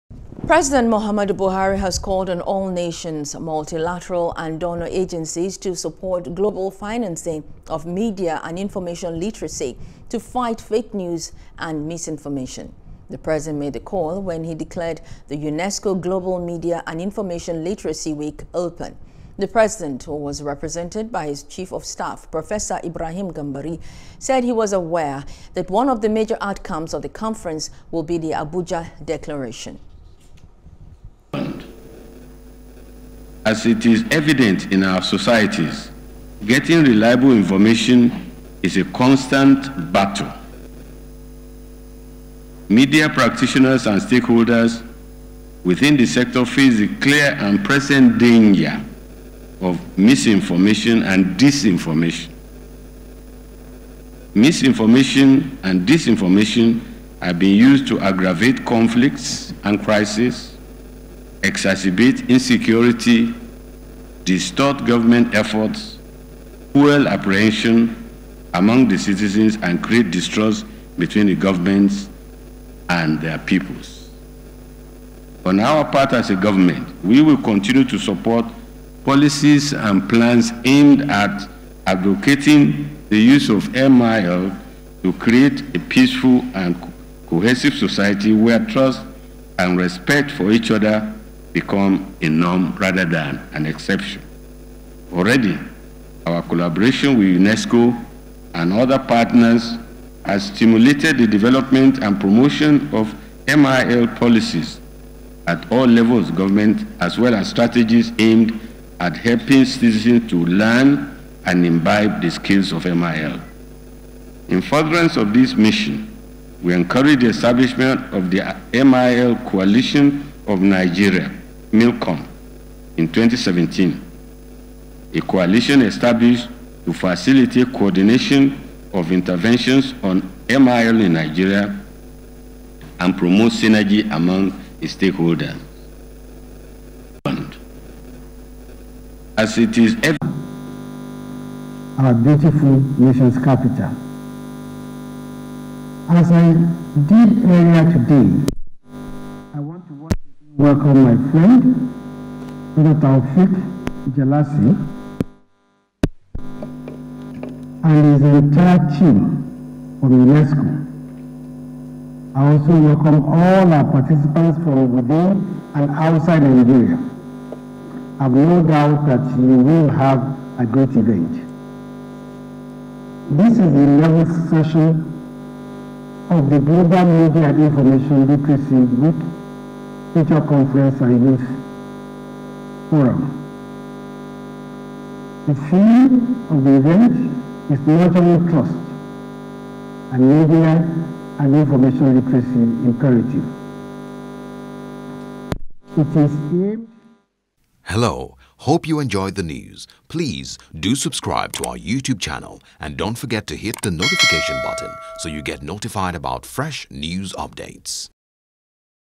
Media Literacy: Buhari Calls For Global Funding To Fight Fake News
President Muhammadu Buhari has called on all nations, multilateral and donor agencies, to support global financing of Media and Information Literacy (MIL) to fight fake news, misinformation.